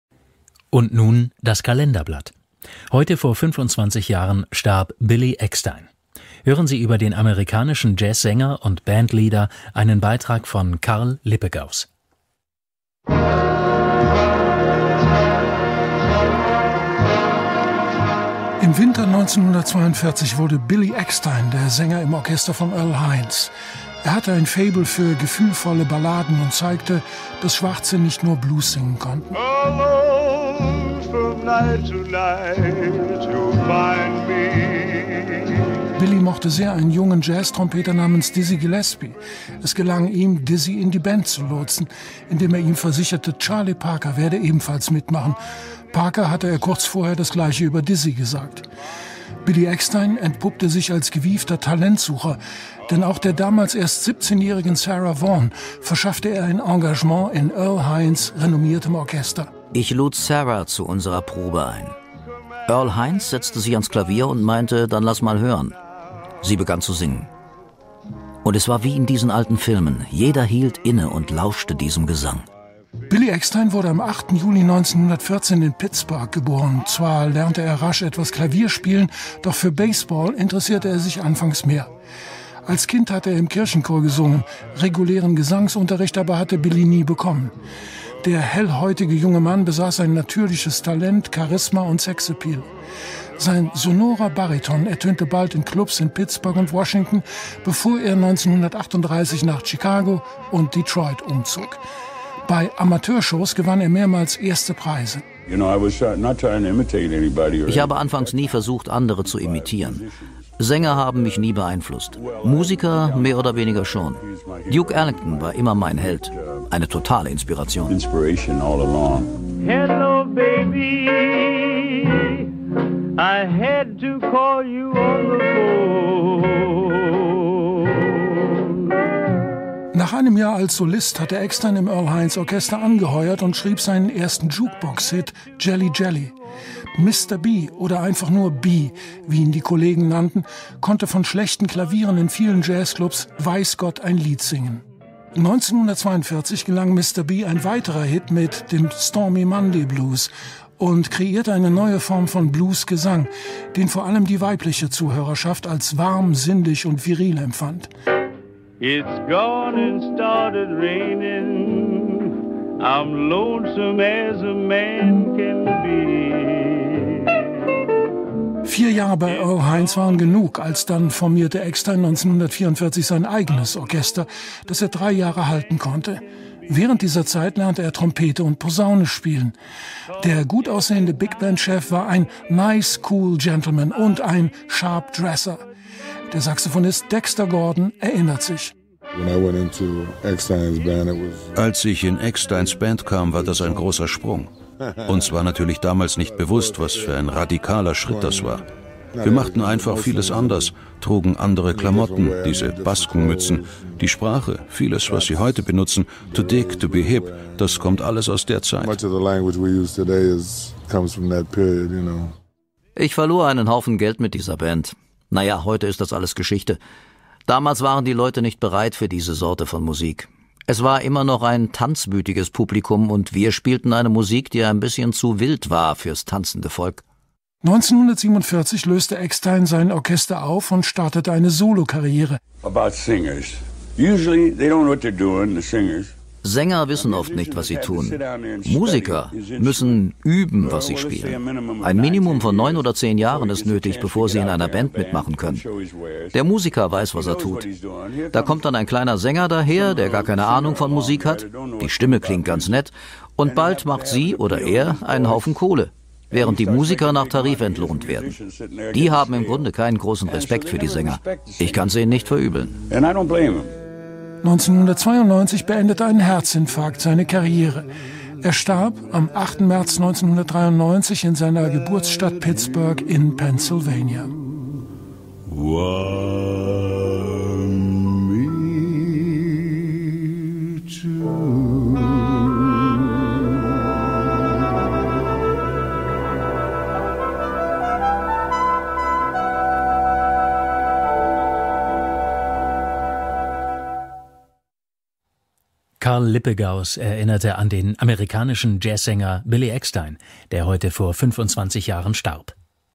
DLF 08.03.2018 Vor 25 Jahren Der Jazzsänger und Bandleader Billy Eckstine gestorben
Als einer der ersten Orchester-Musiker übertrug Billy Eckstine den revolutionären Bebop-Stil aufs Bigband-Format. Mit seiner Baritonstimme bewies Eckstine zudem, dass ein schwarzer Sänger nicht nur als Blues-Sänger, sondern auch als Balladenkünstler langfristig Erfolg haben konnte.
Von Karl Lippegaus